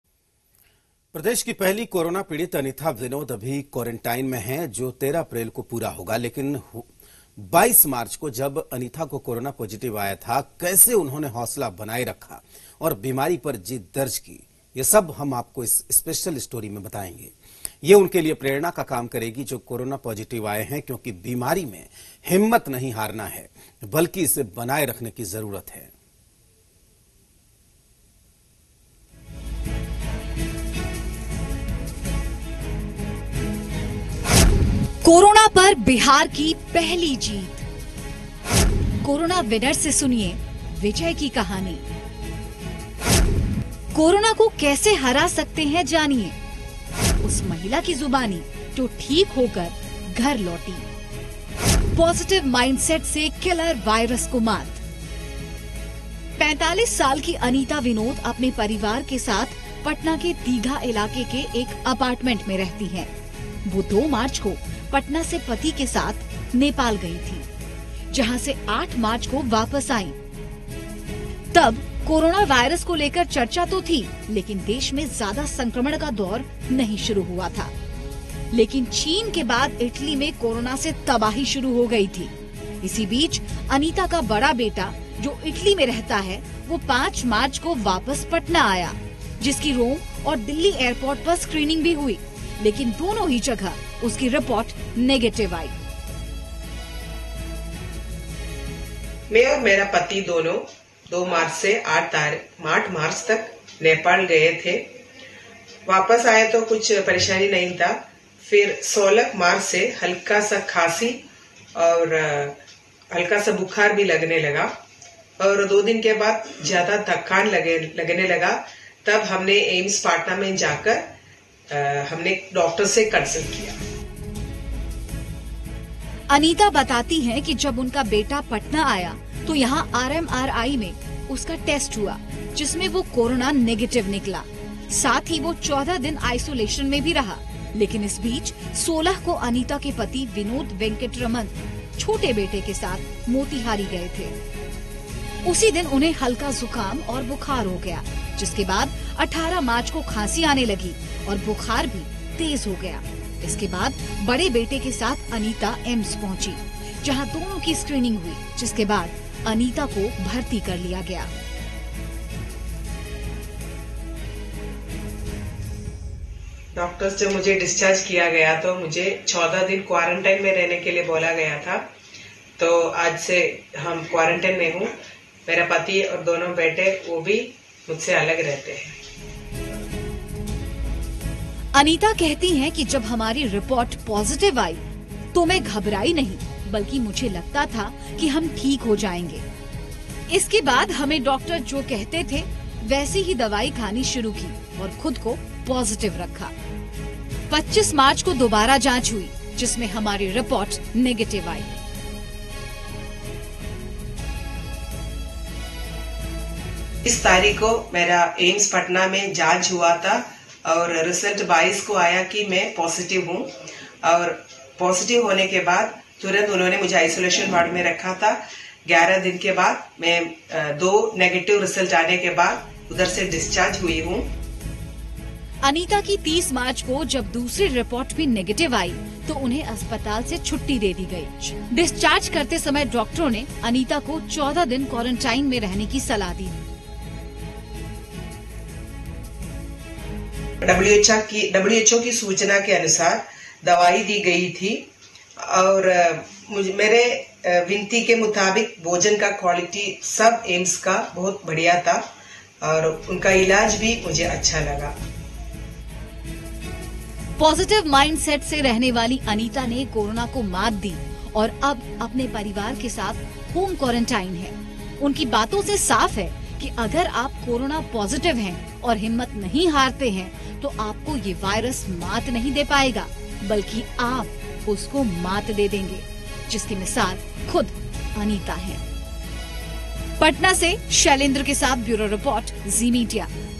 Bihar: corona विनर के सुनिए 'विजय' की कहानी | Bihar Corona News
प्रदेश की पहली कोरोना पीड़ित अनिथा विनोद अभी कोरेंटाइन में हैं, जो 13 अप्रैल को पूरा होगा, लेकिन हुआ 22 मार्च को जब अनिथा को कोरोना पॉजिटिव आया था, कैसे उन्होंने हौसला बनाए रखा और बीमारी पर जीत दर्ज की. ये सब हम आपको इस स्पेशल स्टोरी में बताएंगे, ये उनके लिए प्रेरणा का काम करेगी, जो कोरोना पॉजिटिव आए हैं, क्योंकि बीमारी में हिम्मत हारने नहीं, बल्की बनाए रखने की जरूरत है.